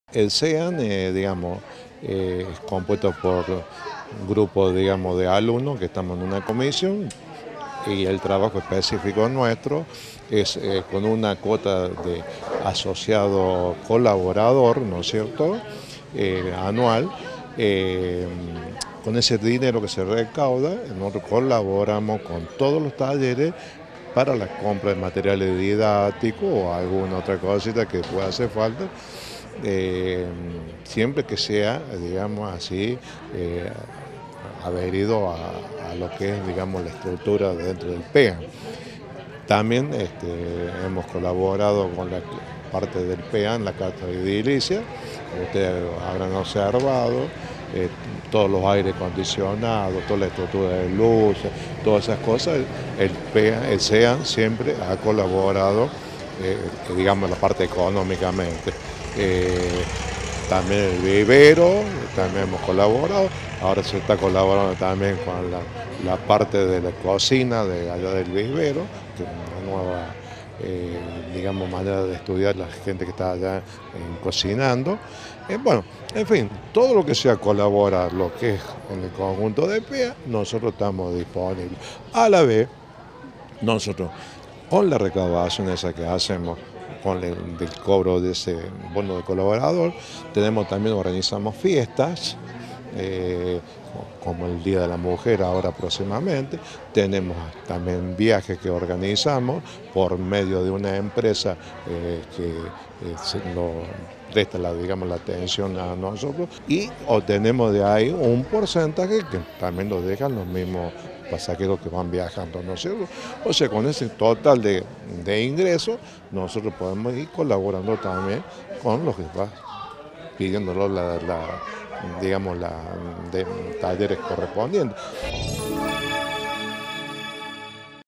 EL CENTRO DE ESTUDIANTES DE ADULTOS MAYORES FORTALECE SU ACTIVIDAD EN EL PEAM
JOSÉ VIDALLE – PRESIDENTE CEAM
Facebook.com/uniriotv
Twitter.com/uniriotv